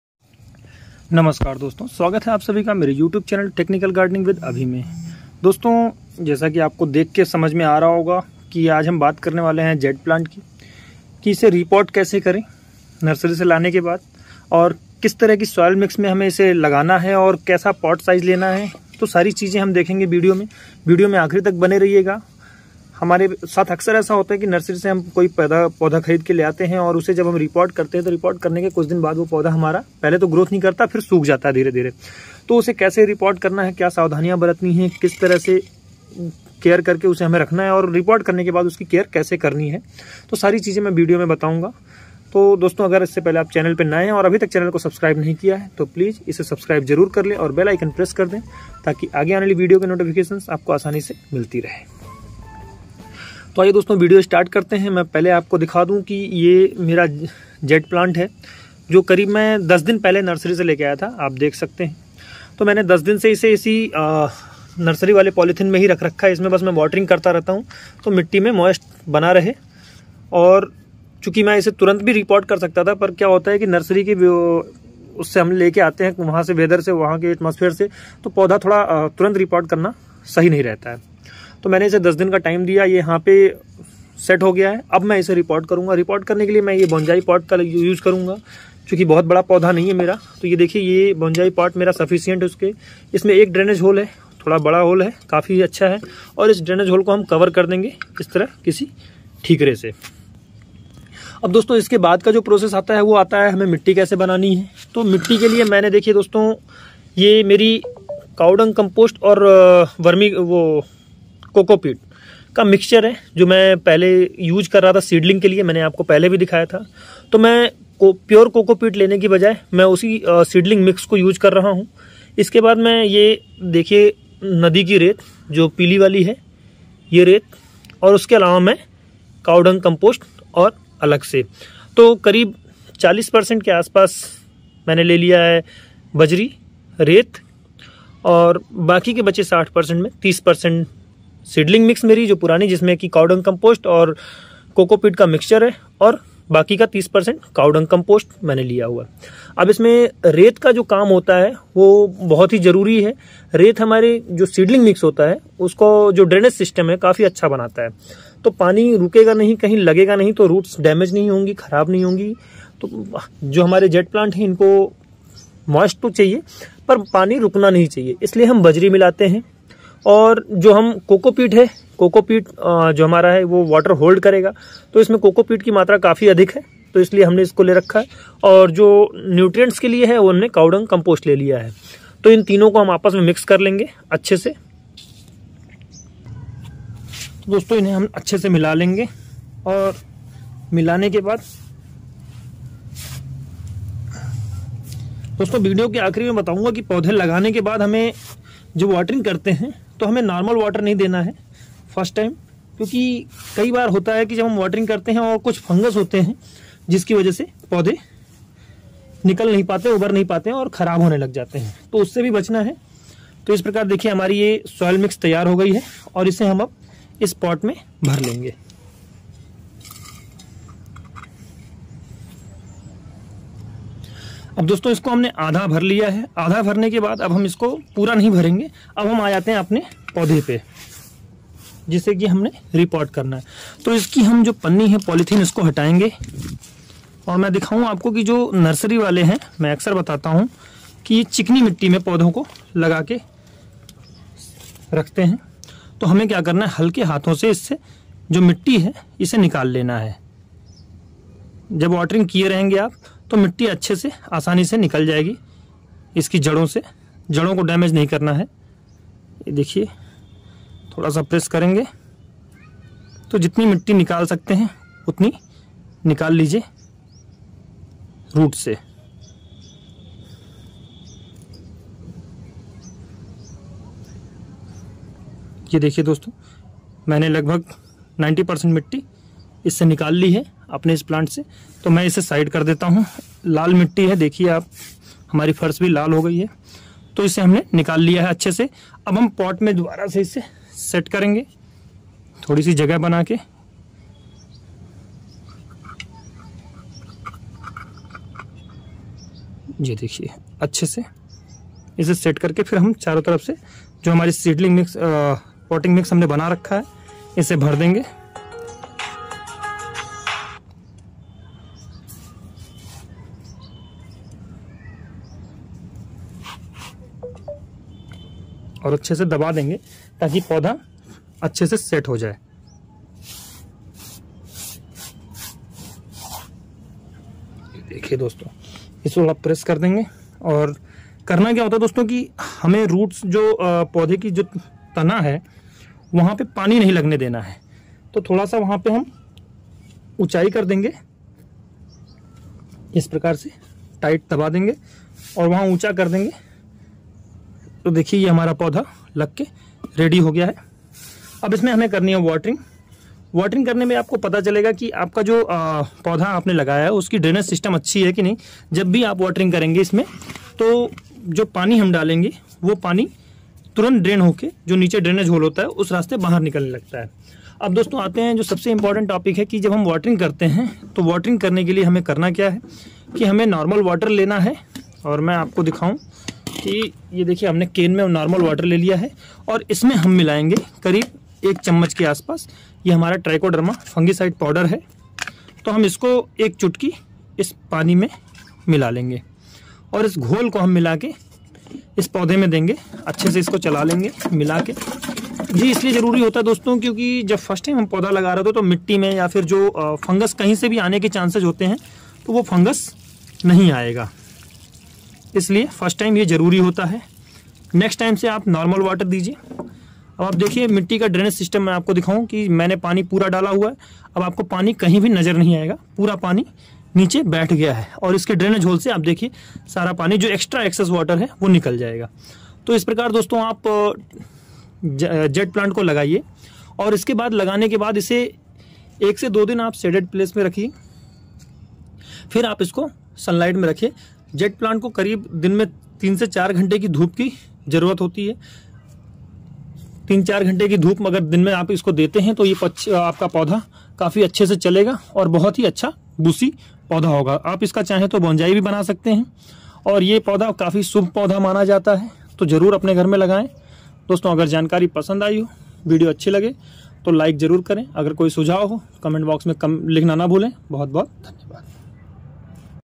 कैसे बनाएं Jade plant के लिए मिट्टी और कैसे करें उसकी देखभाल?
Jade plant ko kaise lagaye aur kaise kare uski dekhbhal?

Magical Fertilizer
Vermi compost
Epsom Salt
Green net